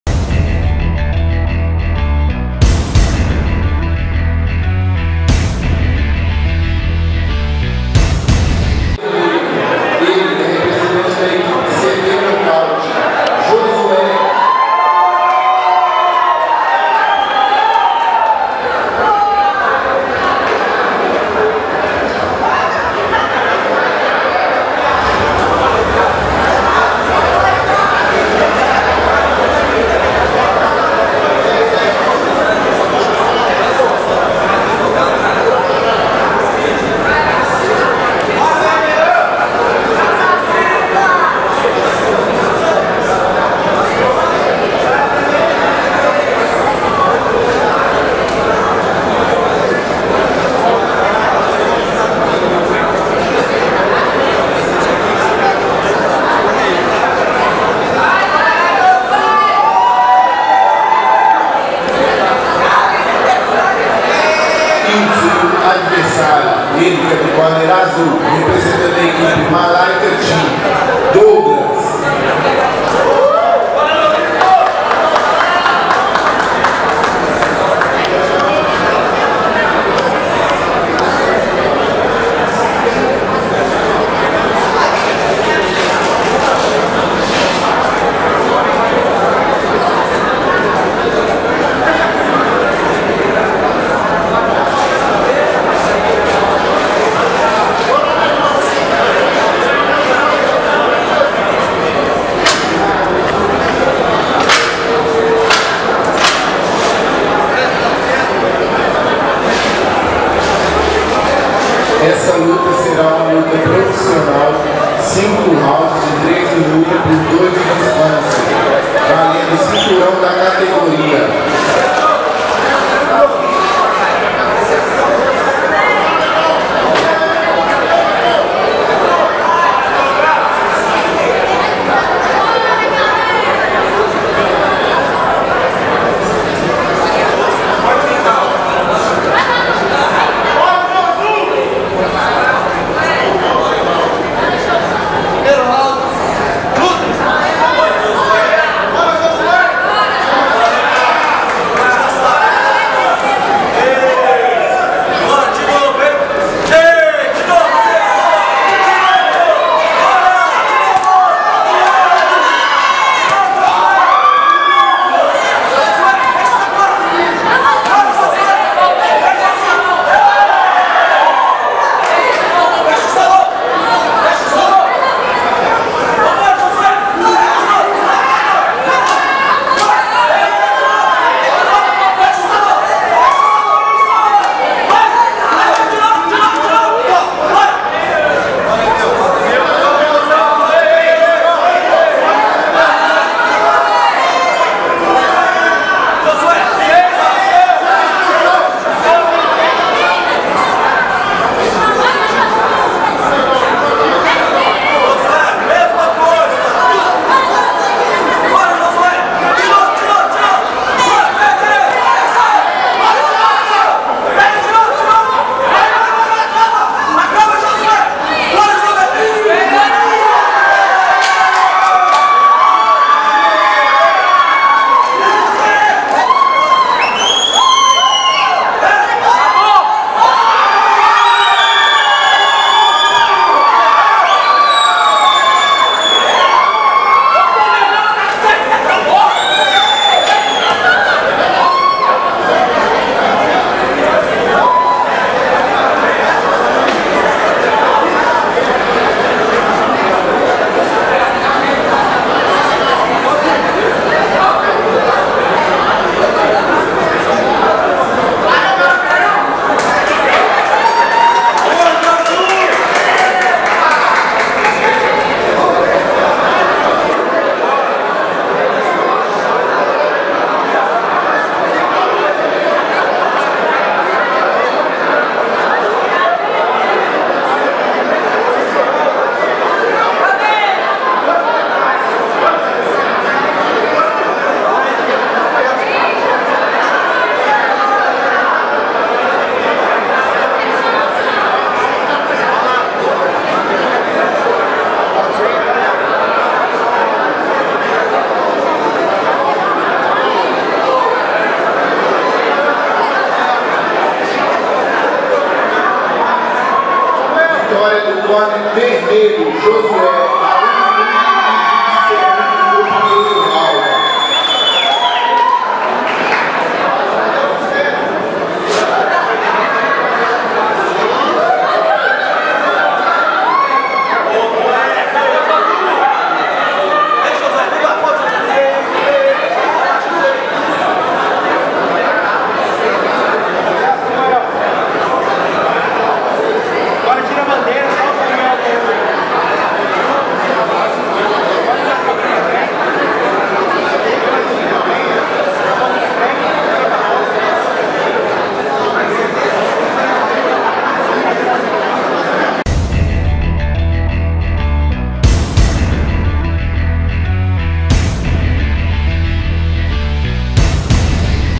Douglas vs Josué - Copa Brutus de Artes Marciais
Em combate de MuayThai profissional até 77kg, Josué Toguro (CT Nocaute) venceu Douglas Soares (Team Malayka) por nocaute no primeiro round e conquistou o cinturão da organização.